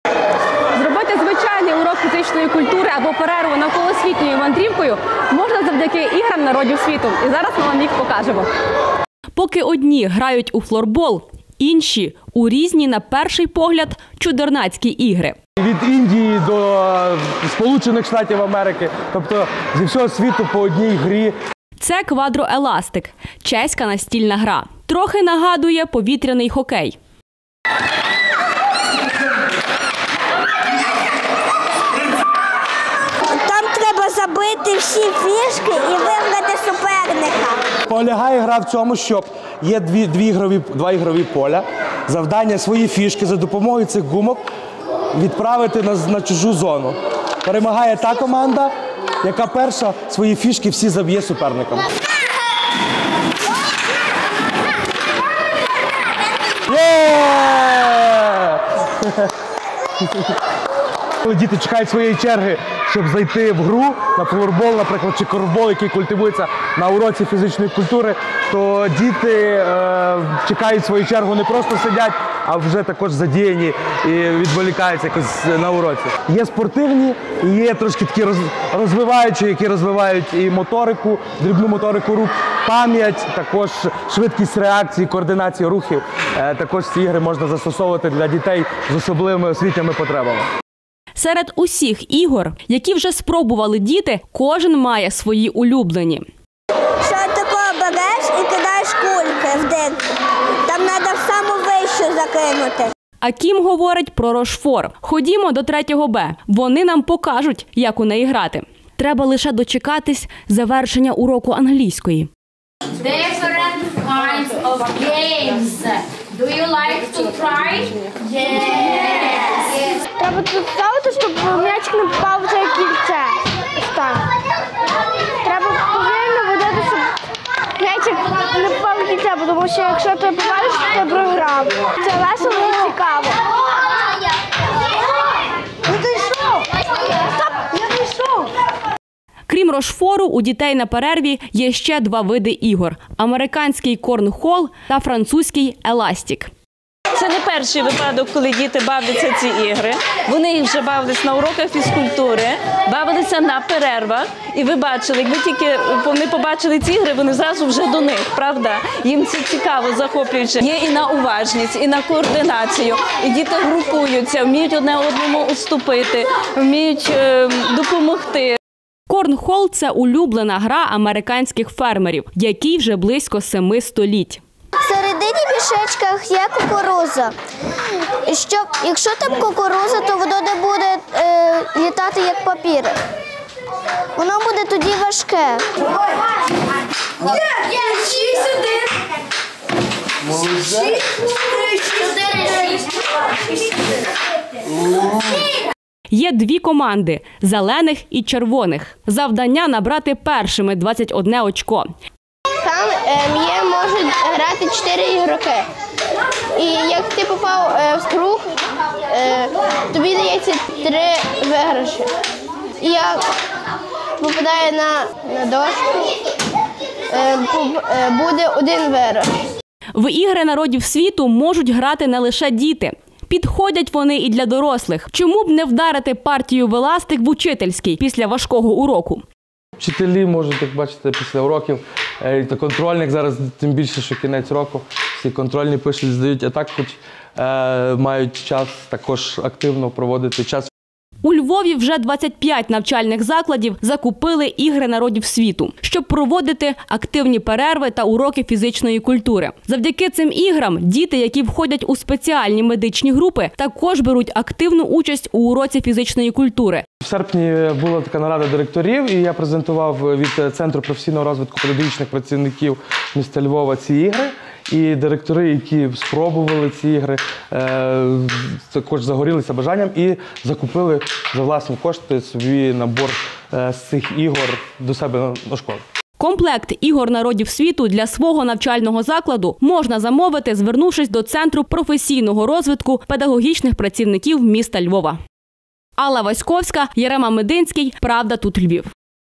Школярам Львівщини запропонували новий вид дозвілля - ігри народів світу
Інше бачення перерв та уроків фізичного виховання у школах. Школярам запропонували новий вид дозвілля - ігри народів світу.

Вже 25 навчальних закладів Львова використовують ці ігри на уроках фізкультури та під час перерв